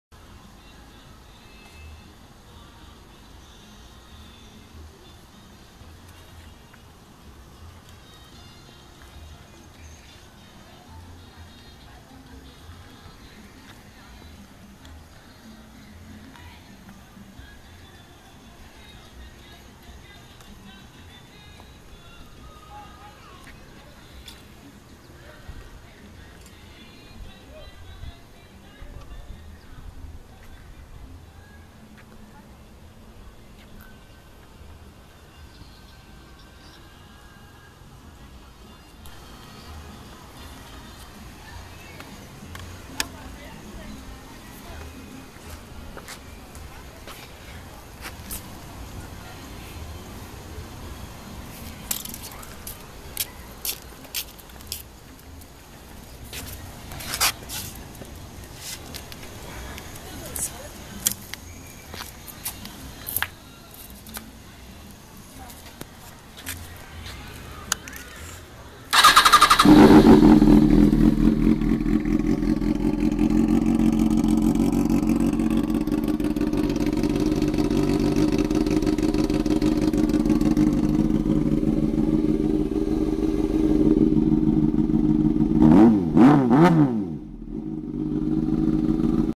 05 ZX6R Walk Around
White 2005 Kawasaki zx6r with red halo lights and crazy pipes.